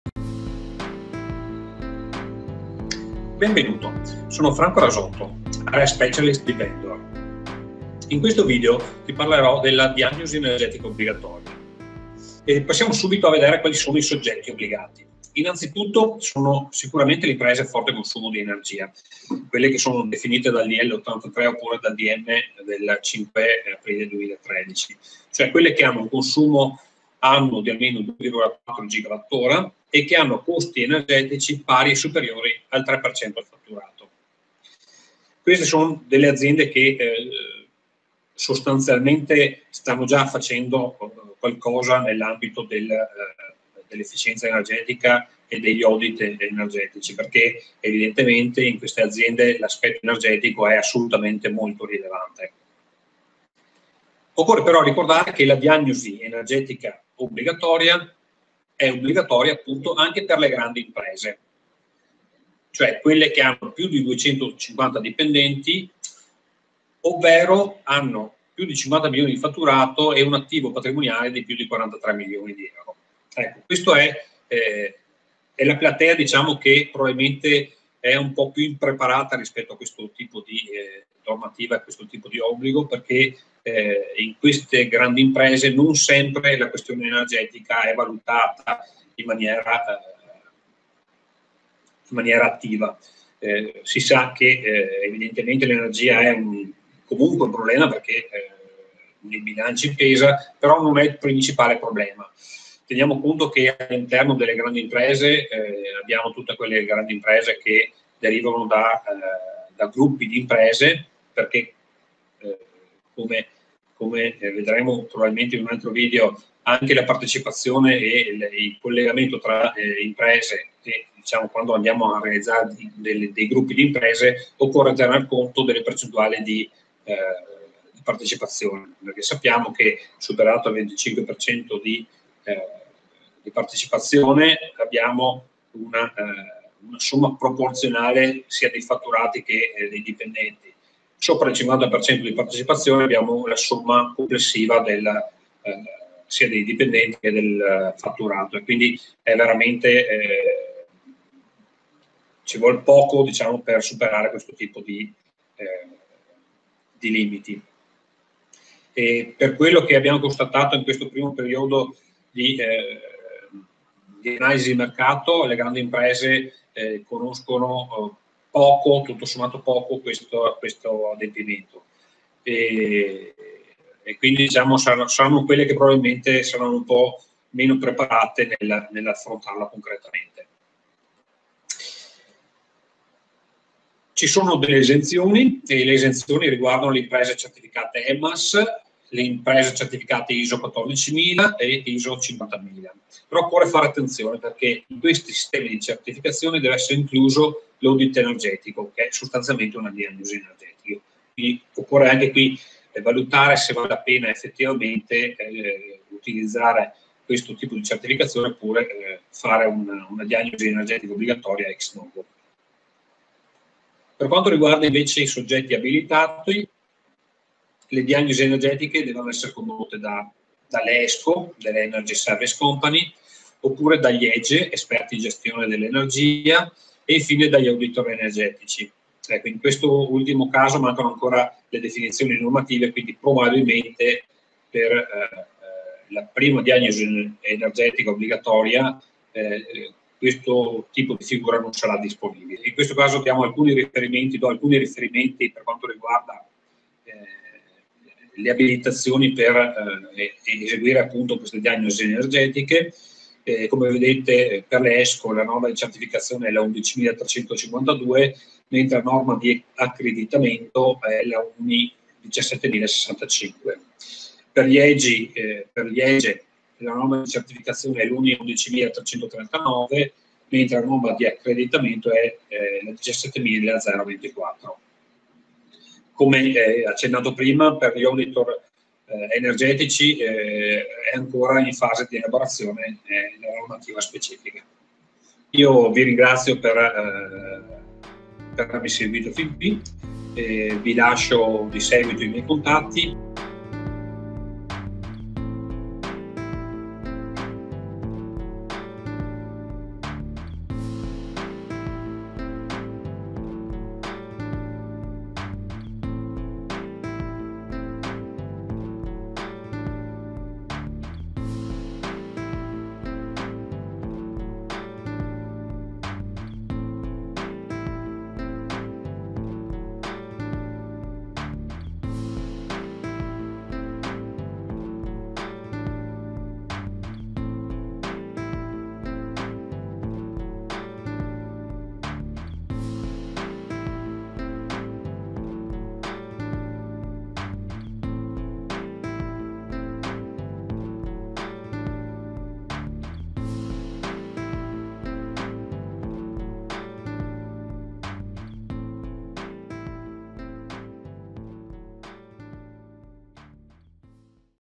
Aziende Obbligate alla Diagnosi Energetica Obbligatoria dlgs 102/14
In questo primo video viene affrontata la questione legata all'individuazione di quali sono le aziende OBBLIGATE ad effettuare la DEO - Diagnosi Energetica Obbligatoria - come previsto dal Dlgs 102/14.
Viene poi affrontata la questione legata ai SOGGETTI ABILITATI alla redazione di questi Audit Energetici.

DEO
Diagnosi Energetica Obbligatoria
Dlgs 102/14